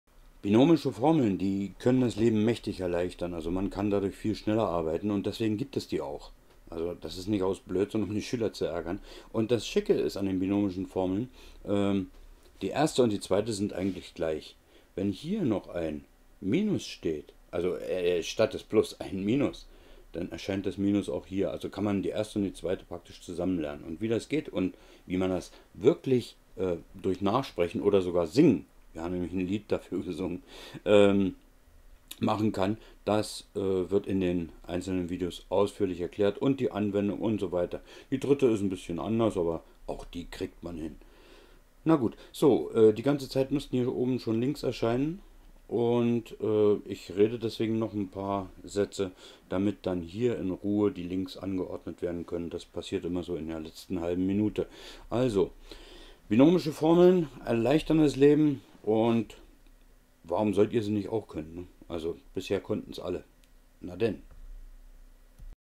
Binomische Formeln. Rechnen damit? Aber wie einfach lernen? Siehe Links in diesem Video!
Bitte, helfen Sie uns Endlich wieder (Sapere aude!) Binomische Formeln sollte man im Schlaf können, weil sie vieles erleichtern (zB. das Ausmultiplizieren von Klammerausdrücken).
Auch erlauben sie die Faktorisierung von Termen, also die Umformung von bestimmten Summen und Differenzen in Produkte, was bei der Vereinfachung von Bruchtermen, beim Radizieren von Wurzeltermen sowie Logarithmenausdrücken sehr oft die einzige Lösungsstrategie darstellt.
Eigentlich sie Spezialfälle des Distributivgesetzes für algebraische Summen (jedes Glied der einen wird mit jedem der anderen Summe multipliziert). (Quelle: Wikipedia)